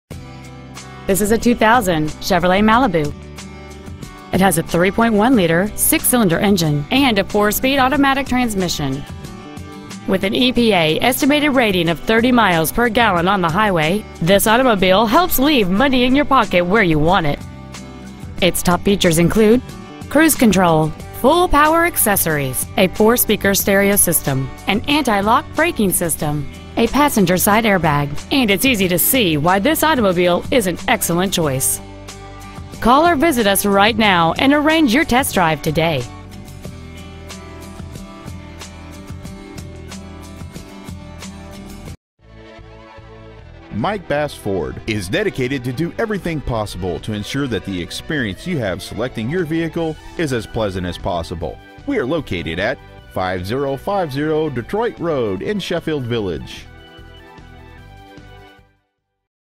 Preowned 2000 CHEVROLET MALIBU Sheffield Village OH
Year: 2000
 Make: CHEVROLET
 Model: MALIBU
 Engine: 3.1L V6
 Trans.: AUTO 4SPD
 Exterior: GRAY
 Miles: 106,422
 Interior: TAN

 Used 2000 CHEVROLET MALIBU Sheffield Village  OH